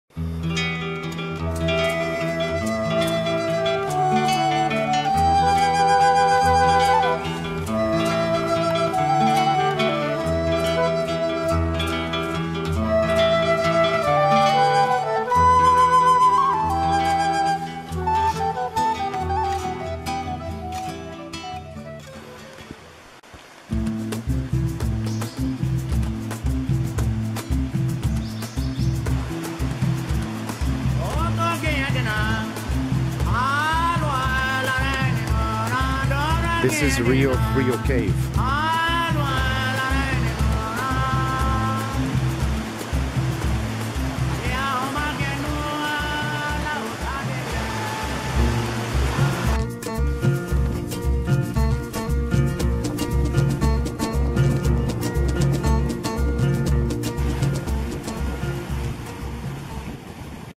Belize Caracol Mayan Ruin Tour Package - Bzmtours
Caracol displays the classic rain forest and jungle vegetation which is home to a wide variety of flora and fauna.

Belize Magnificent Mayan Tours Caracol Tour Package includes other sites like Rio on Pools and  Rio Frio Cave